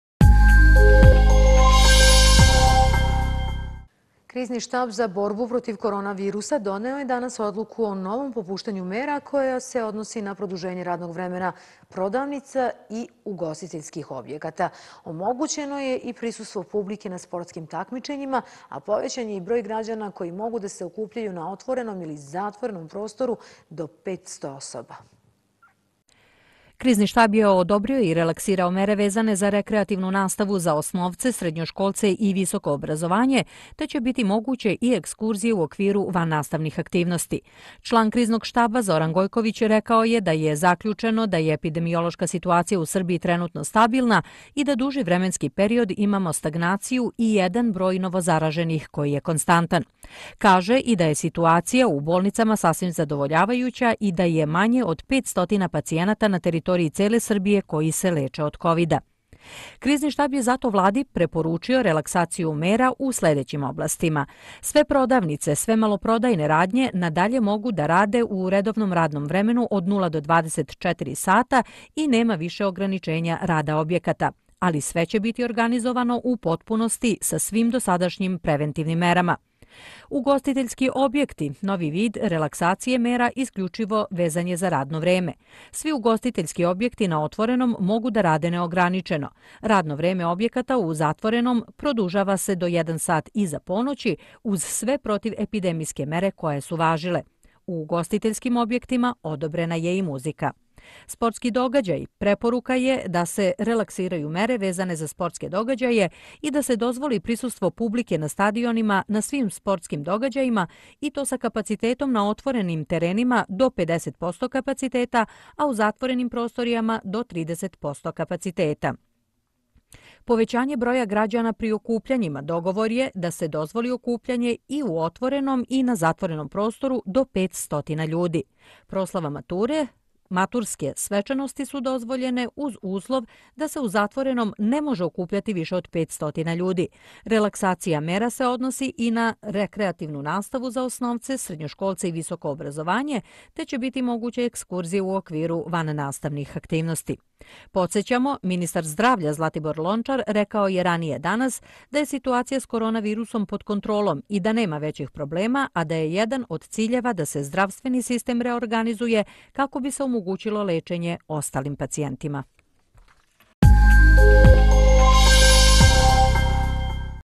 Krizni štab Odobrene mature i slavlja, dozvoljena publika na stadionima
Krizni štab za borbu protiv koronavirusa doneo je danas odluku o novom popuštanju mera koja se odnosi na produženje radnog vremena prodavnica i ugostiteljskih objekata. Omogućeno je i prisustvo publike na sportskim takmičenjima, a povećan je i broj građana koji mogu da se okupljaju na otvorenom ili zatvorenom prostoru do 500 osoba.